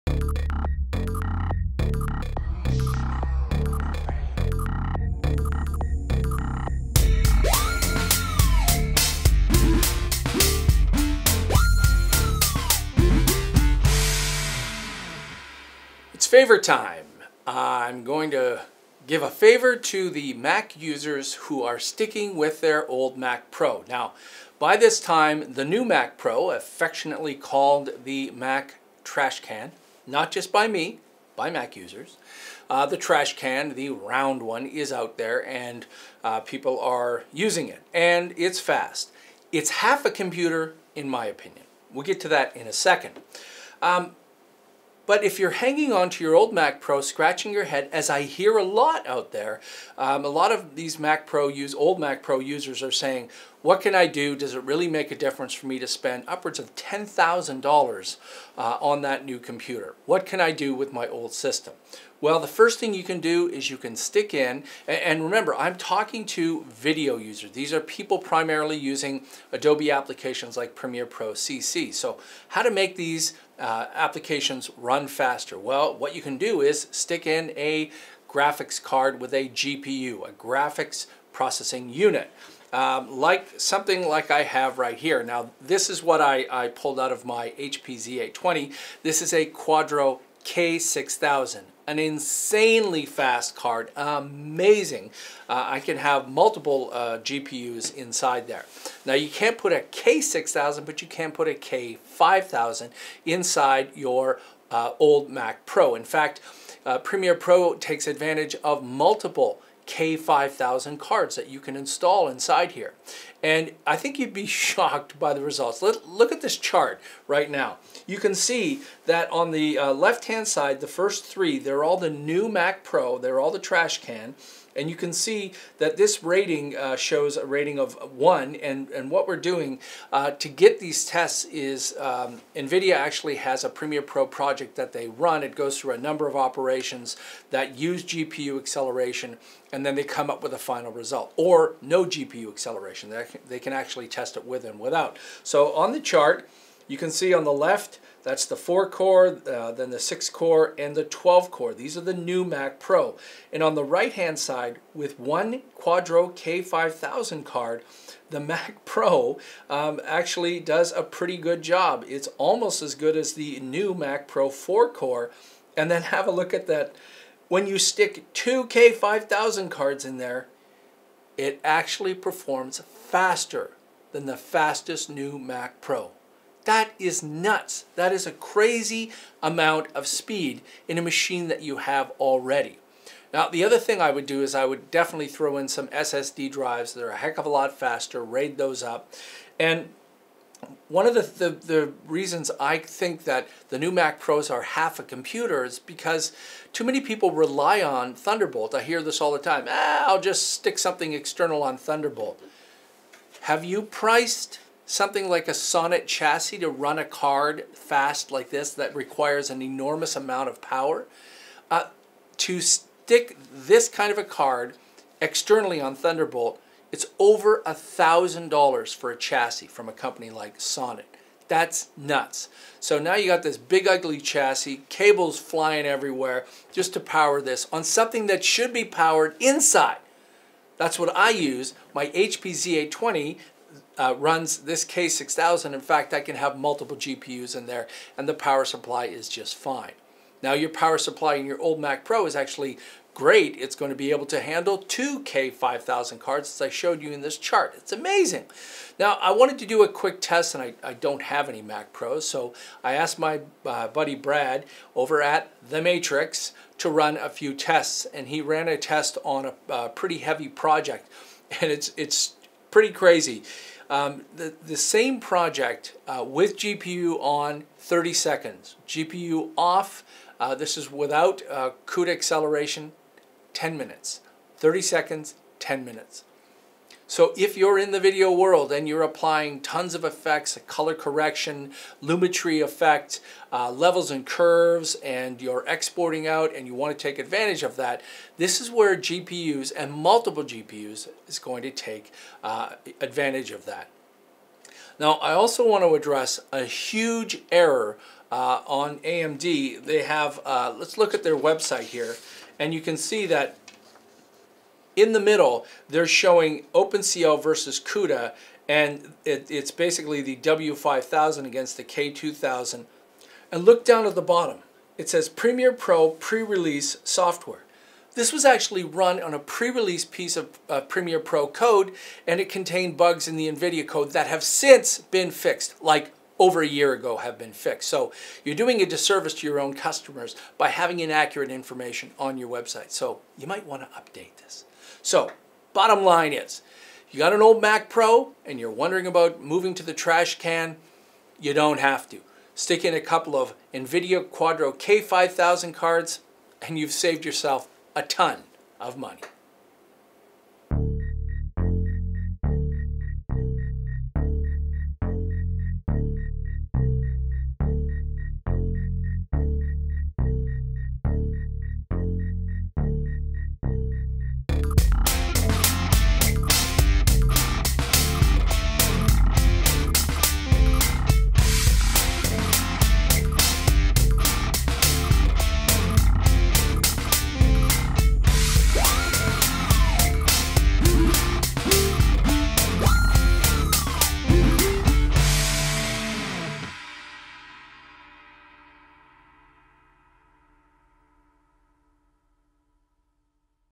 NVIDIA Quadro breaths new life into your old MacPro!
If you are still a Mac user and you're wondering if you should shell out a ton of money on the new MacPro (Trash Can) this episode will show you that, with the addition of a couple of K5000 cards, you may already have a faster Mac!